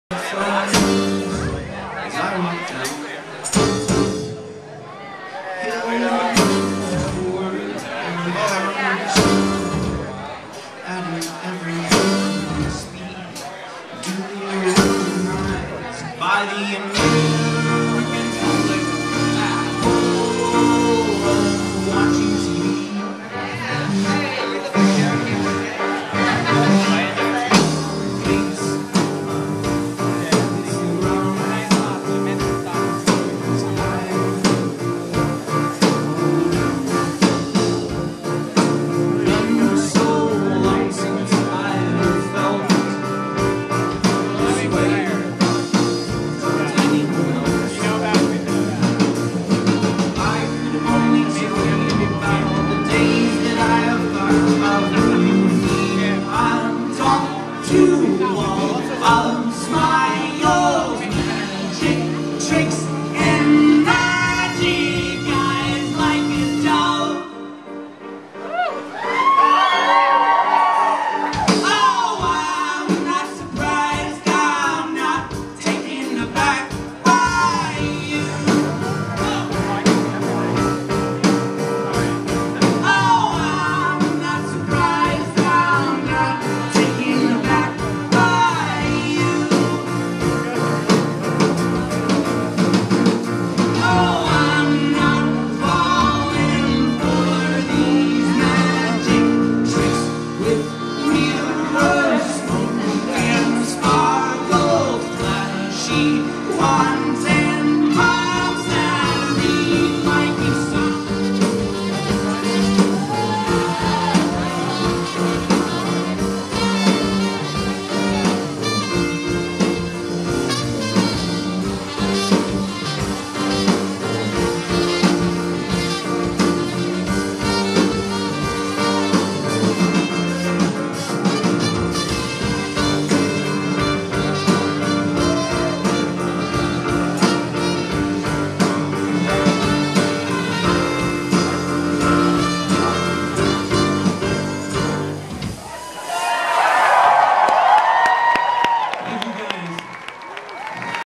Campfire OK at the Capitol Hill Block Party
Campfire OK playing Neumos at the Capitol Hill Block Party